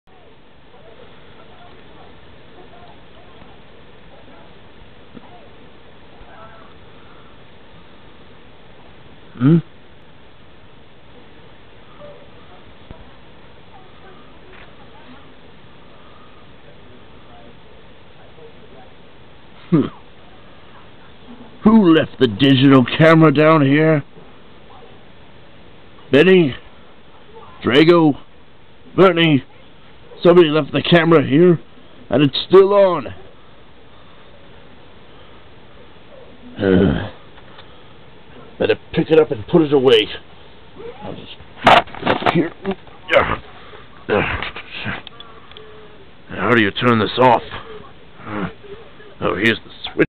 Miller finds the camera left on
Miller finds the camera left on and unattended and turns it off. Pretty simple sounding, but just watch!